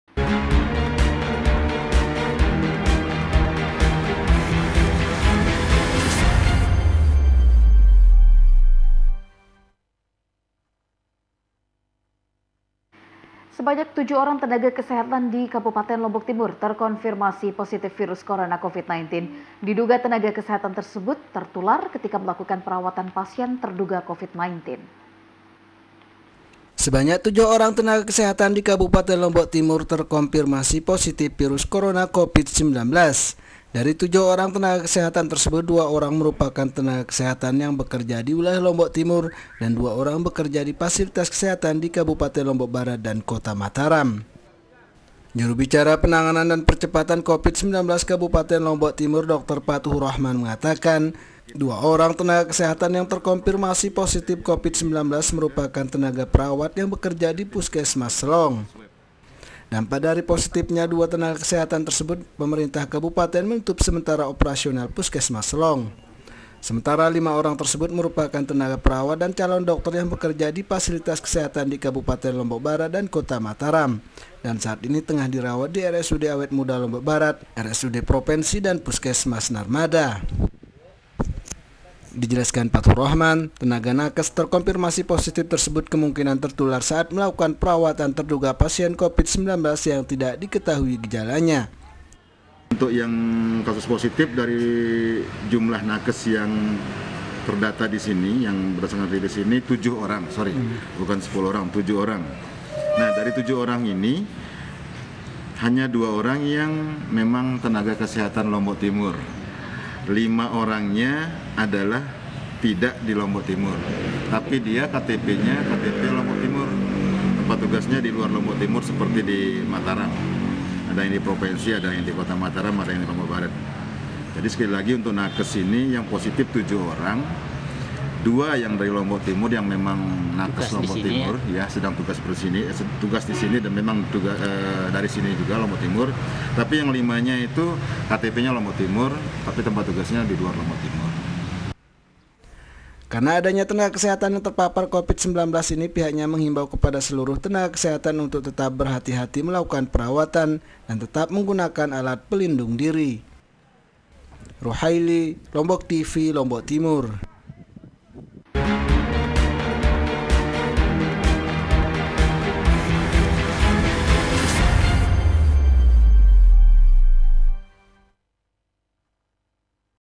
Tujuh Orang Nakes Lotim Positif Covid 19
Nuansa Lombok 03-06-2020  Tujuh Orang Nakes Lotim Positif Covid 19